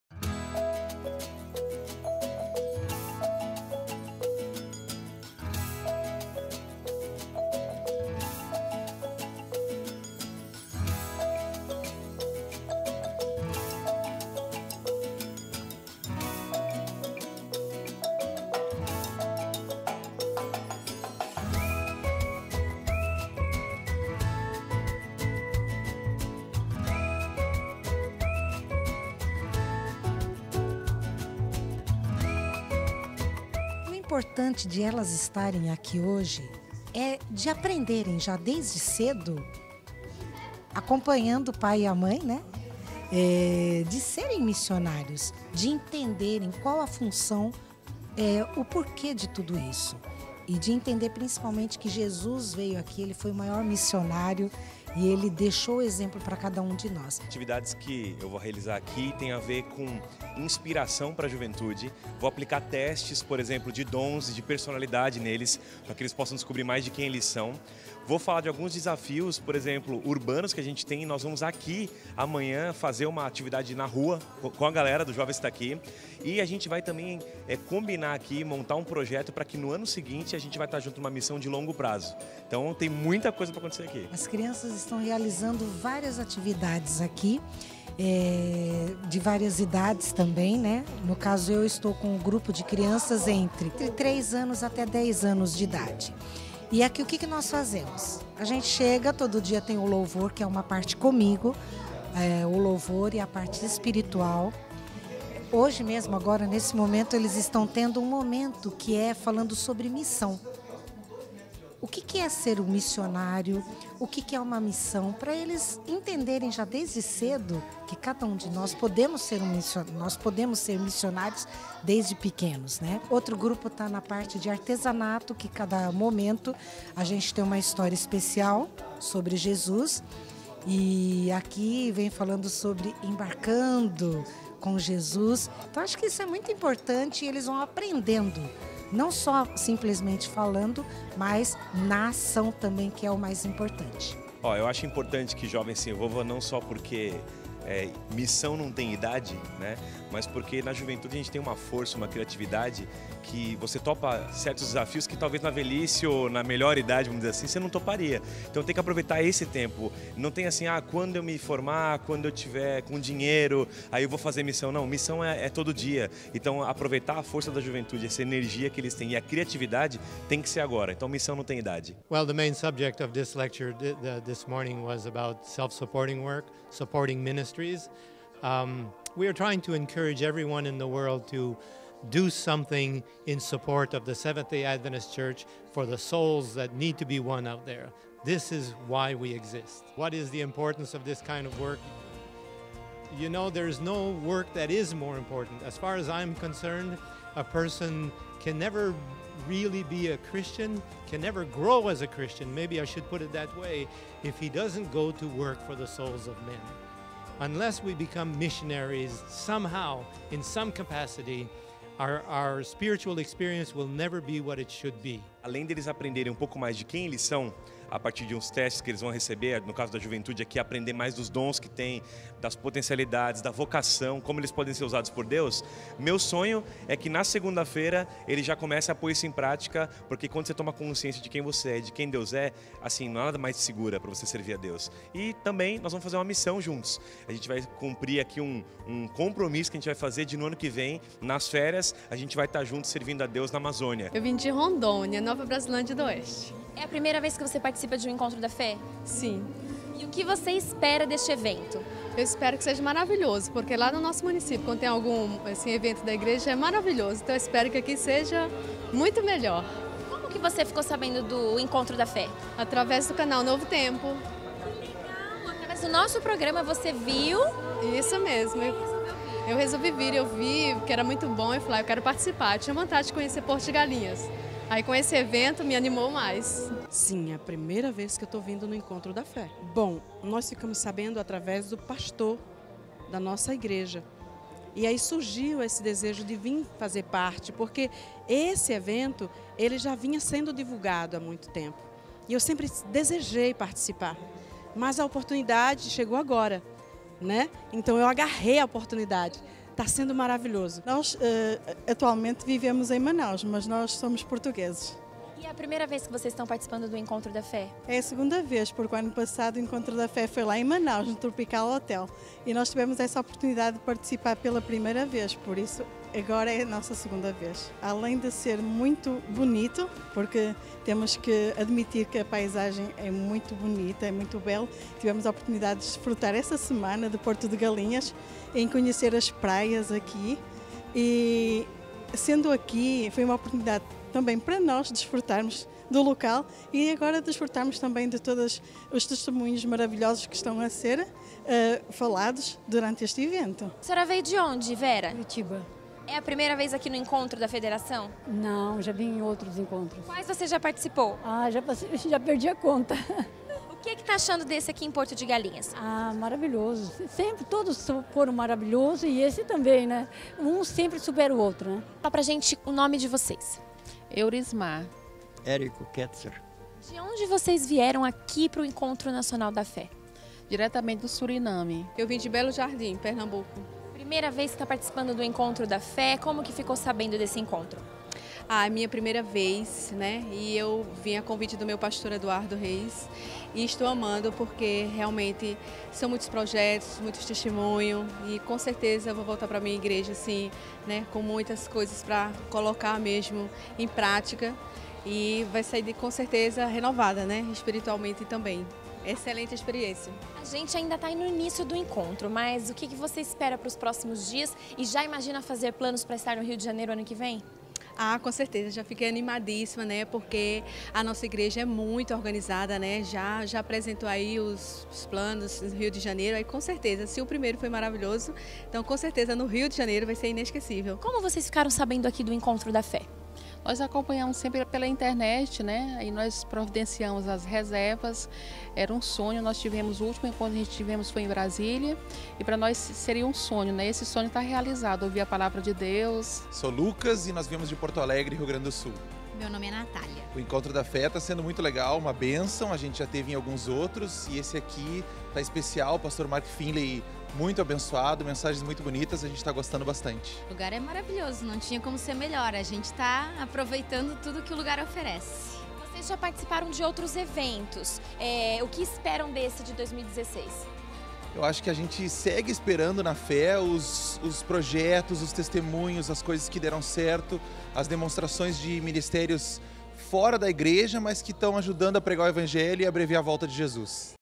Reportagem 2 Encontro da FE - parte II
Direto de Pernambuco, Porto de Galinhas, nossa equipe vai te mostrar tudo o que aconteceu no 27º Encontro Nacional dos Empreendedores Adventistas do Brasil. Você vai entender a importância da Federação para a Igreja Adventista e para a sociedade. E também confere a programação especial desenvolvida com crianças e jovens, no Encontro que reúne toda família com um único objetivo: desenvolver o espírito de missão.

Programa exibido em 25/set/2016.